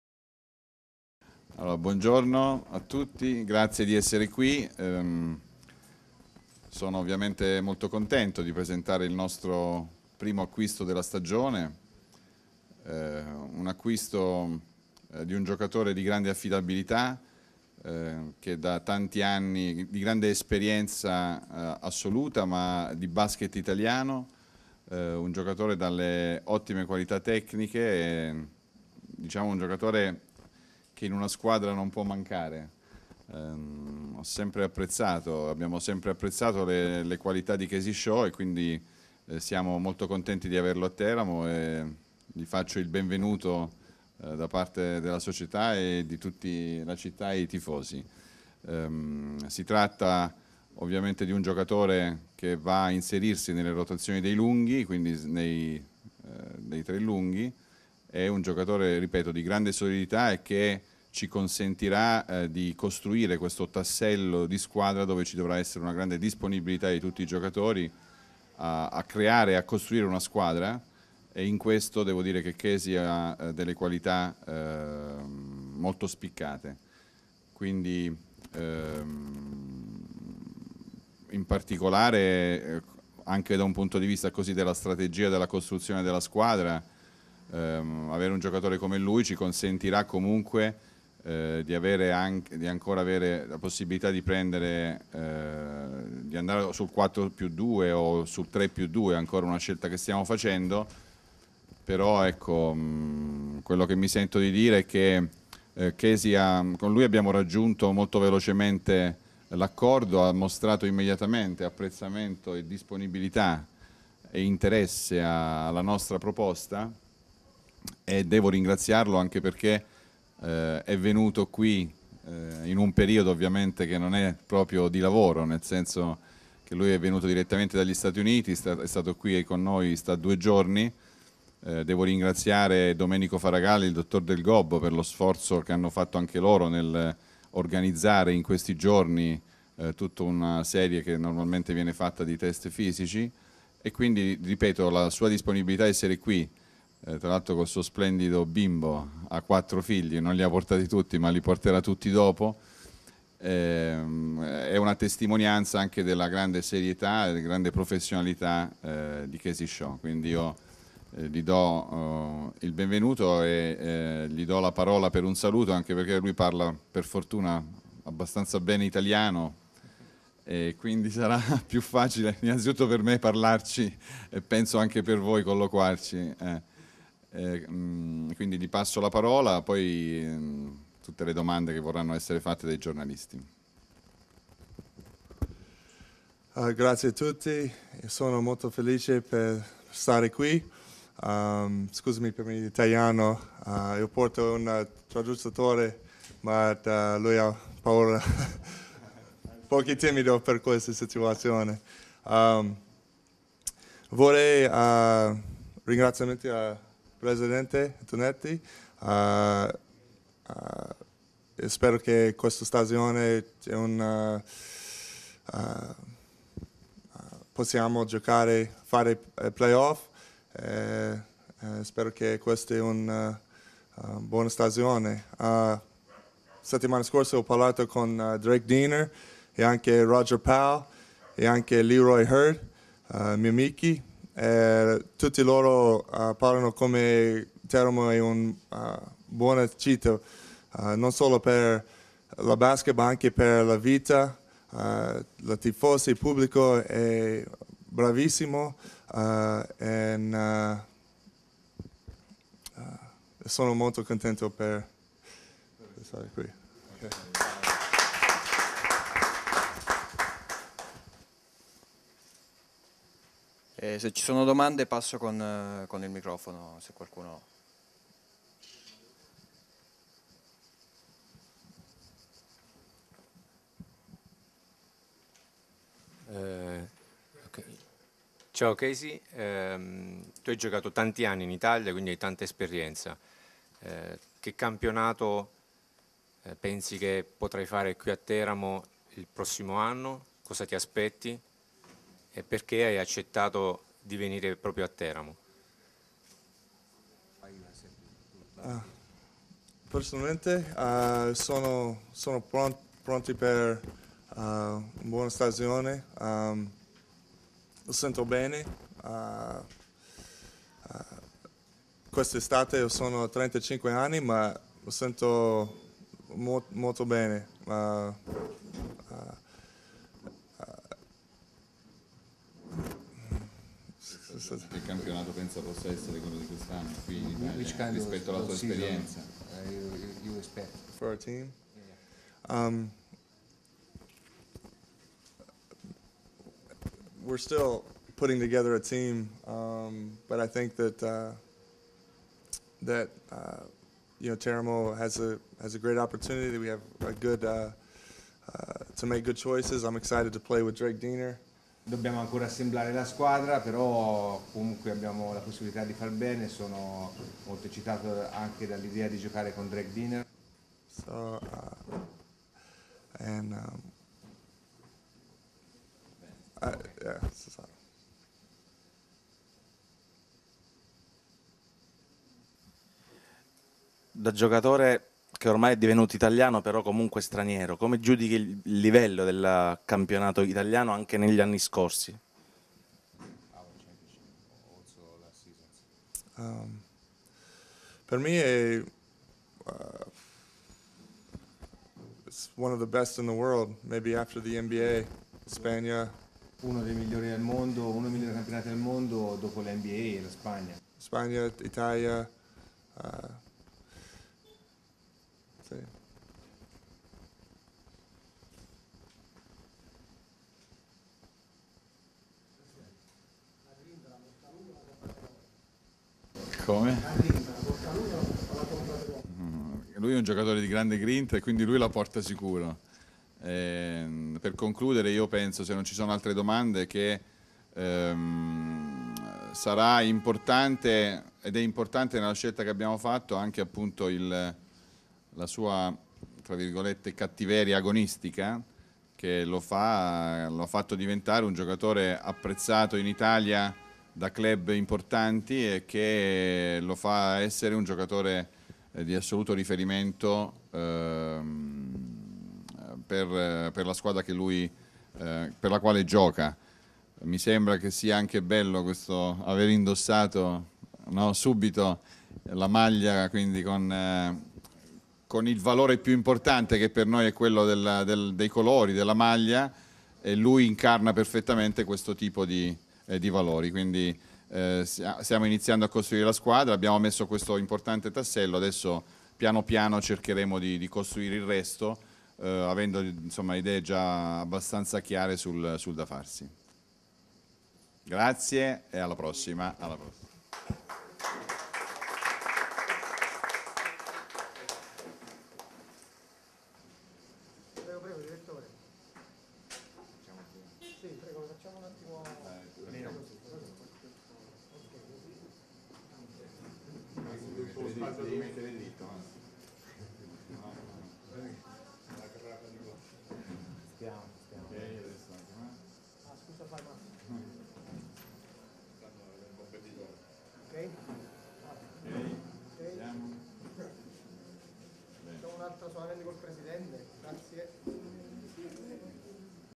Presentazione Casey Shaw
Prima operazione in entrata per la Banca Tercas Teramo che si è assicurata le prestazione del veterano Casey Shaw, statunitense con passaporto italiano. Il giocatore è stato presentato nella sede sociale della Teramo Basket